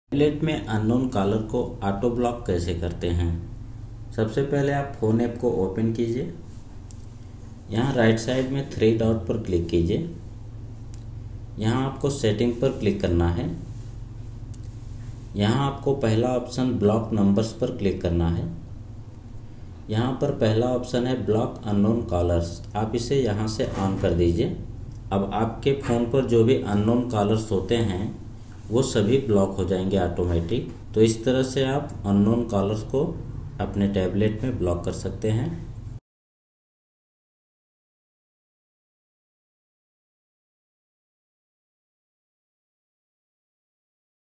How to block Unknown number in Android Tablet? Tablet me Unknown number ko block kaise karte hai?
How to block Unknown number in Android Tablet?
Android Tablet me Unknown number ko block kaise karte hai?
ऐंड्रॉड टैब्लेट में अन्नोन नम्बर को ब्लॉक कैसे करते है?

कोई सवाल पूछने के लिए instagram और फेसबुक पेज पर मेसेज भेजे।